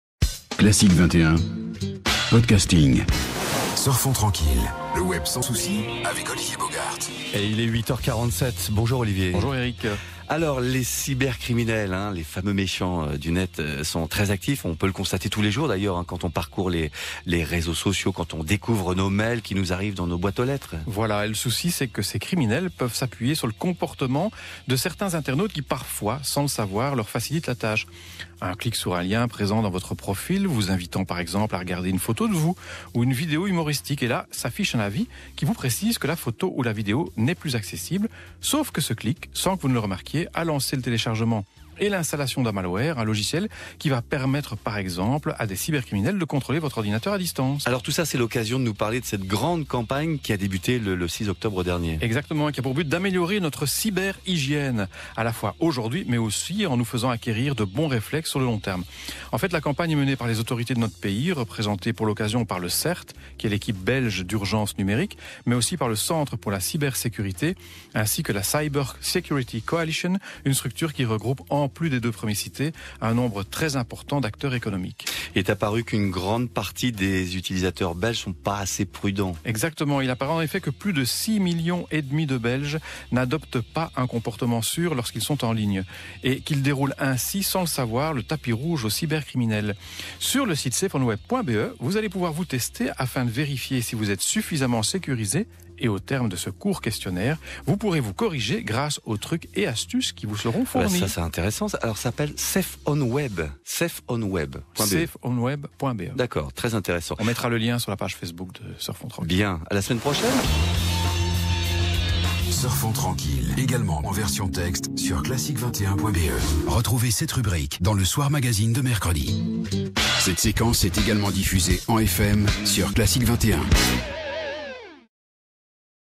Surfons Tranquille:La campagne pour une cyber-hygiène
Le souci, c'est que ces criminels peuvent s'appuyer sur le comportement de certains internautes qui parfois, sans le savoir, leur facilitent la tache.

Un clic sur un lien présent dans votre profil vous invitant à regarder une photo de vous ou une vidéo humoristique. Et là, s'affiche un avis qui vous précise que la photo ou la vidéo n'est plus accessible.

Sauf que ce clic, sans que vous ne le remarquiez, a lancé le téléchargement et l'installation d'un malware, un logiciel qui va permettre, par exemple, à des cybercriminels de contrôler votre ordinateur à distance.

L'occasion de vous parler de cette grande campagne qui a débuté le 6 octobre dernier et qui a pour but d'améliorer notre cyber-hygiène à la fois aujourd'hui mais aussi en nous faisant acquérir de bon réflexes sur le long terme.

Cette campagne est menée par les autorités de notre pays représentées, pour l'occasion, par le CERT qui est l'équipe belge d'urgence numérique mais aussi par le Centre pour la Cybersécurité ainsi que la Cyber Security Coalition, une structure qui regroupe, en plus des deux premiers cités, un nombre très important d'acteurs économiques.

Et tous ont décidé de cette campagne parce qu'il est apparu qu'une grande majorité d'utilisateurs belges ne sont pas assez prudents.

Il apparaît en effet que près de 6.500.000 belges n'adoptent pas un comportement sûr lorsqu'ils sont en ligne sûr et qu'ils déroulent ainsi, sans le savoir, le tapis rouge aux cybercriminels.

Sur le site safeonweb.be, vous allez pouvoir vous tester afin de vérifier si vous êtes suffisamment sécurisé et, au terme de ce court questionnaire, vous pourrez vous corriger grâce aux trucs et astuces qui vous seront fournis.

Veilig Surfen: Een campagne voor meer cyberhygiëne

Wanneer u gebruikmaakt van de sociale media of de mails in uw inbox leest, kunt u dag na dag vaststellen dat cybercriminelen bijzonder actief zijn.

Het onrustwekkende is dat die criminelen profiteren van het gedrag van bepaalde internetgebruikers die hen soms onbewust helpen bij het plegen van misdrijven.

Via een link in uw profiel kunt u een foto van uzelf of een humoristisch filmpje bekijken. Als u hierop klikt, krijgt u het bericht dat de foto of het filmpje niet meer beschikbaar is.

Hierdoor hebt u echter ongemerkt een malwareprogramma gedownload en geïnstalleerd waarmee cybercriminelen bijvoorbeeld op afstand controle hebben over uw computer.

Dit bewijst het nut van de grote campagne die op 6 oktober 2016 van start ging en tot doel heeft niet alleen uw 'cyberhygiëne' van het moment te verbeteren, maar ook u de juiste reflexen aan te leren op lange termijn.

Deze campagne wordt gevoerd door de overheden van ons land, vertegenwoordigd door het Belgische CERT (Computer Emergency Response Team) alsook door het Centrum voor Cybersecurity België en de Cyber Security Coalition, een partnership tussen de eerste twee organen en een groot aantal economische actoren.

Samen hebben ze het initiatief tot deze campagne genomen omdat een groot aantal Belgische internetgebruikers onvoorzichtig blijken te zijn.

Zo'n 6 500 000 Belgen surfen immers onveilig en geven zo onbewust cybercriminelen de vrije hand.

Op de website safeonweb.be kunt u aan de hand van een vragenlijst nagaan of u voldoende bent beveiligd en daarna uw surfgedrag bijsturen dankzij de tips en tricks die er worden gegeven.